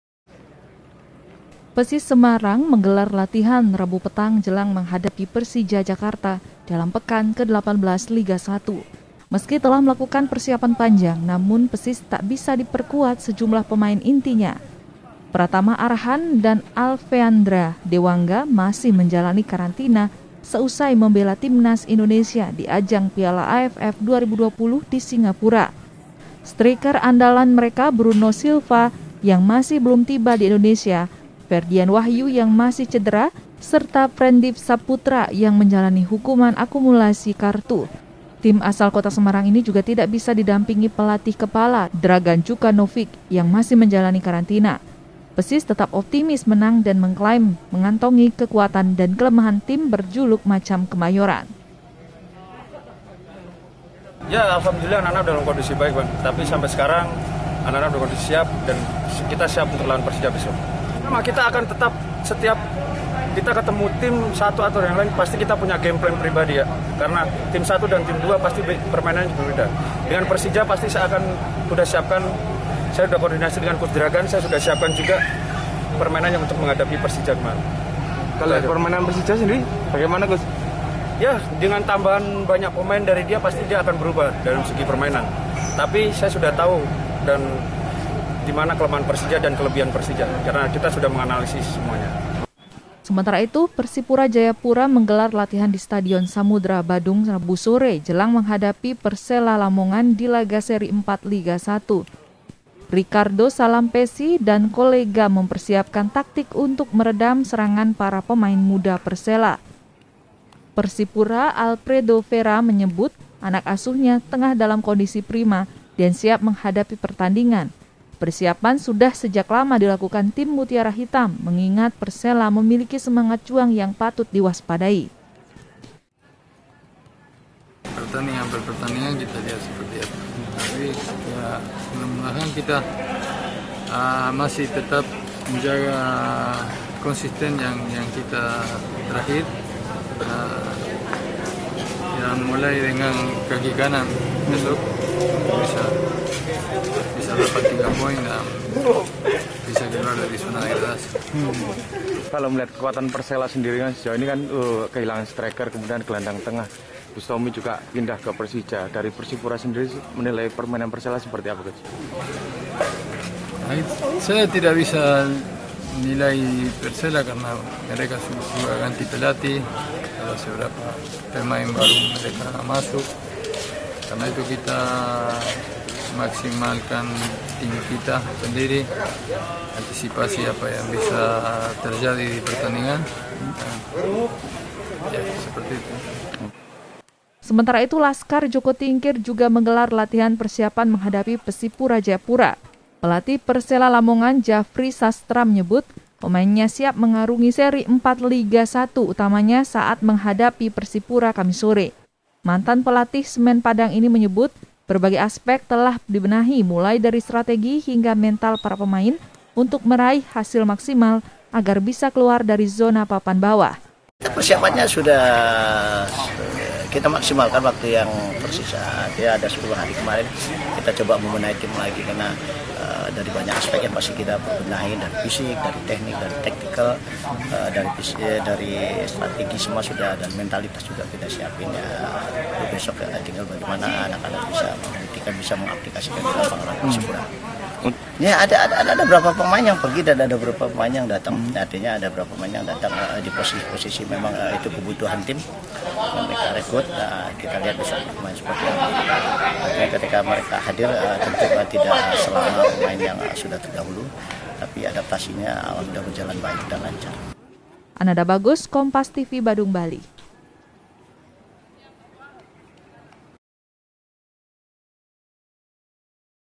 Kantongi Kelemahan Persija, PSIS Optimis Menang
BADUNG, KOMPAS TV - PSIS Semarang menggelar latihan, Rabu (5/1/2022) petang jelang menghadapi Persija Jakarta, dalam pekan ke 18 Liga 1 BRI. Meski telah melakukan persiapan panjang, namun PSIS tak bisa diperkuat sejumlah pemain intinya. Pratama Arhan dan Alfeandra Dewangga masih menjalani karantina seusai membela Timnas Indonesia di ajang piala AFF 2020 di Singapura. Sedangkan striker andalan mereka Bruno Silva yang masih belum tiba di Indonesia, Fredyan Wahyu yang masih cedera, serta Frendi Saputra yang menjalani hukuman akumulasi kartu.  Tim asal kota Semarang ini juga tidak bisa didampingi pelatih kepala Dragan Djukanovic yang masih menjalani karantina. PSIS tetap optimis menang dan mengklaim mengantongi kekuatan dan kelemahan tim berjuluk macan kemayoran.

Sementara itu, Persipura Jayapura menggelar latihan di stadion Samudera Badung, Rabu (1/1/2022) sore, jelang menghadapi Persela Lamongan di laga seri 4 Liga 1 BRI. Ricardo Salampessy dan kolega mempersiapkan taktik untuk meredam serangan para pemain muda Persela. Pelatih Persipura Alfredo Vera menyebut, anak asuhnya tengah dalam kondisi prima, dan siap menghadapi pertandingan. Persiapan sudah sejak lama dilakukan tim mutiara hitam, mengingat persela memiliki semangat juang yang patut diwaspadai.

Sementara itu laskar joko tingkir juga menggelar latihan persiapan menghadapi Persipura Jayapura. Pelatih Persela Lamongan, Jafri Sastra menyebut, pemainnya siap mengarungi seri 4 Liga 1 BRI utamanya saat menghadapi Persipura Kamis (6/1/2022) sore. Mantan pelatih Semen Padang ini menyebut berbagai aspek telah dibenahi, mulai dari strategi, hingga mental para pemain, untuk meraih hasil maksimal agar bisa keluar dari zona papan bawah.